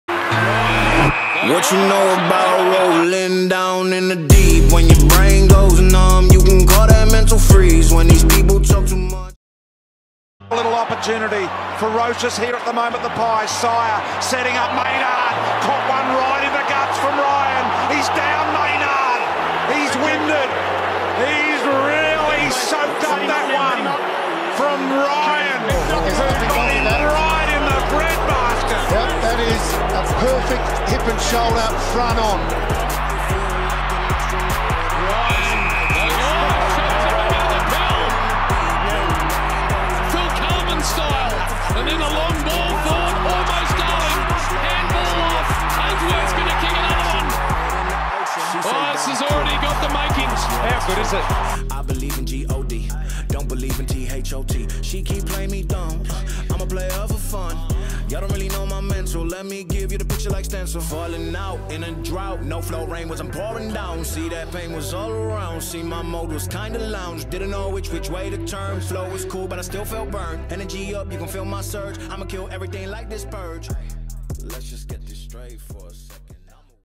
MAYNARD V RYAN - Modern Day Rivalry
Just a muck about video really, let me know if you think that the rivalry will continue. Proper upload coming on friday.